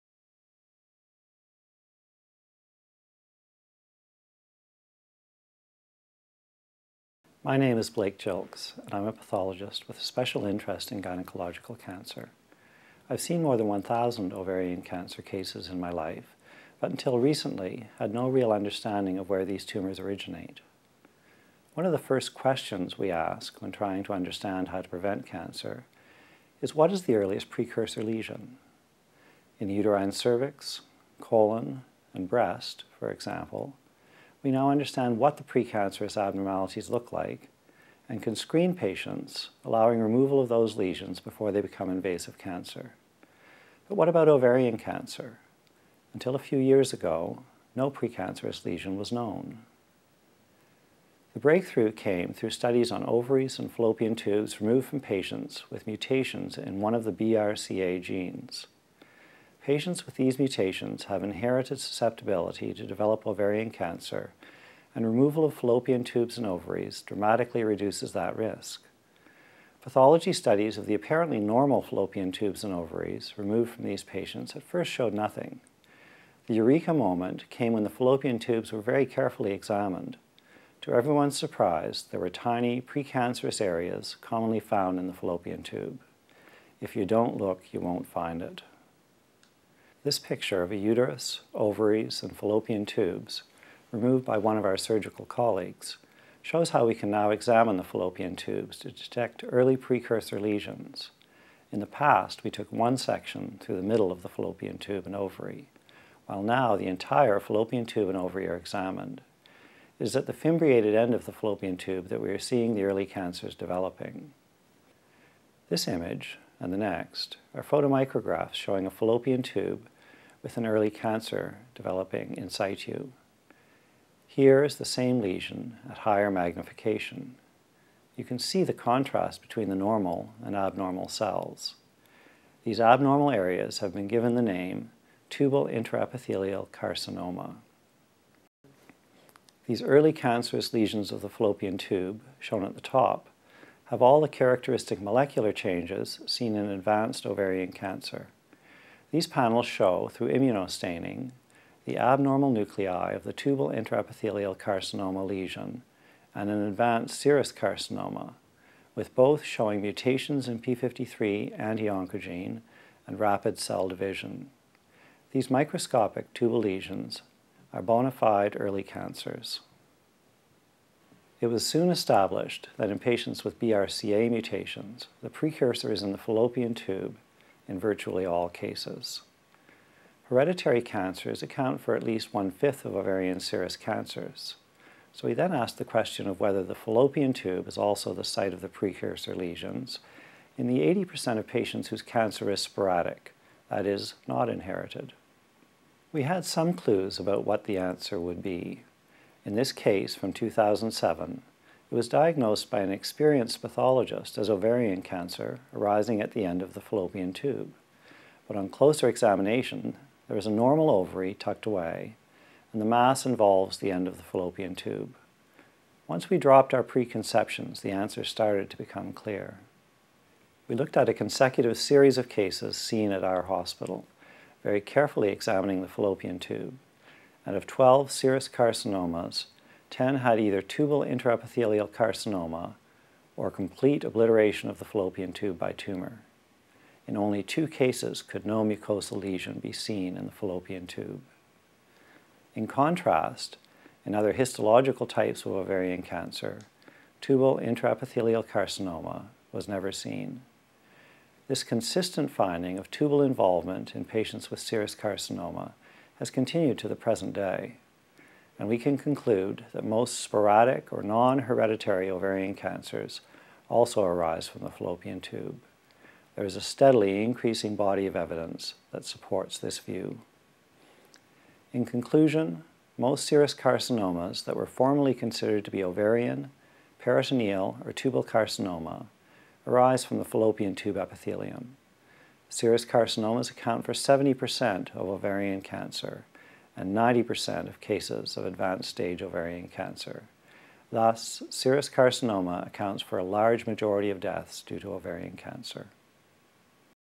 Pathology of Ovarian Cancer
Dr. Blake Gilks, Pathologist